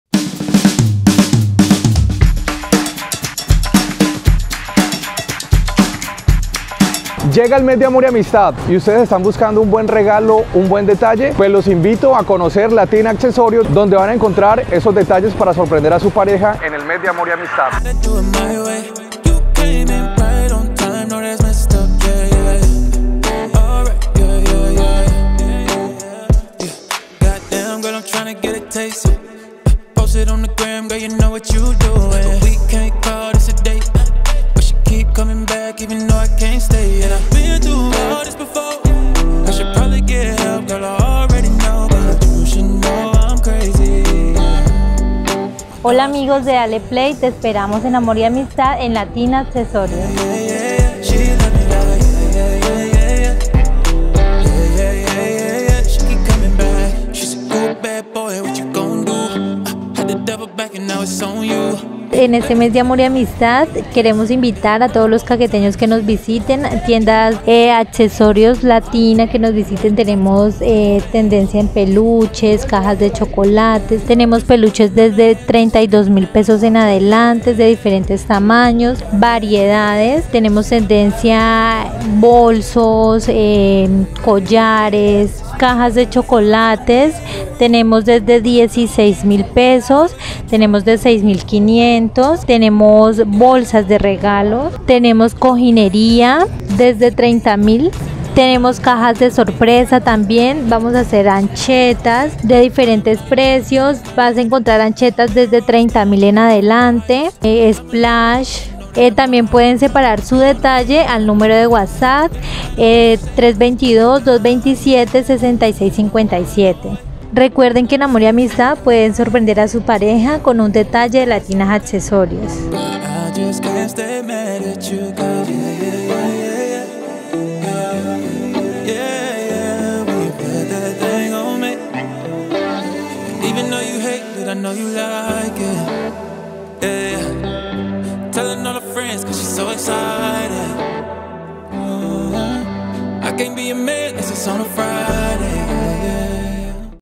Regalos para Amor y Amistad - Peluches, Canastas Decorativas y mas!
Familia Dale Play, en este Mes de Amor y Amistad llegaron a LATINAS ACCESORIOS diversos artículos para entregar en este Mes tan especial. Visítanos y separa tu detalle

Lo que esta de Moda